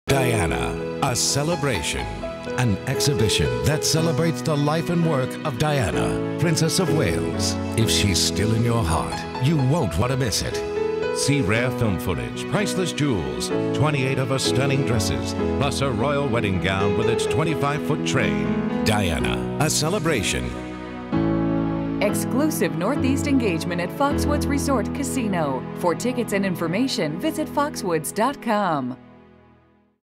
Diana: A Celebration
Foxwoods is proud to play host to the exclusive Northeast engagement of this exhibit.  (September 16 - January 15, 2012 - Great Cedar Exhibition Hall)

Rarely is an exhibition as remarkable as the life which lies behind it.  Direct from the Althorp Estate in England, the ancestral home of the Spencer Family, comes more than 150 personal objects celebrating the life and work of Diana, Princess of Wales, including her royal wedding gown, 28 designer dresses, family heirlooms, personal mementos, and rare home movies.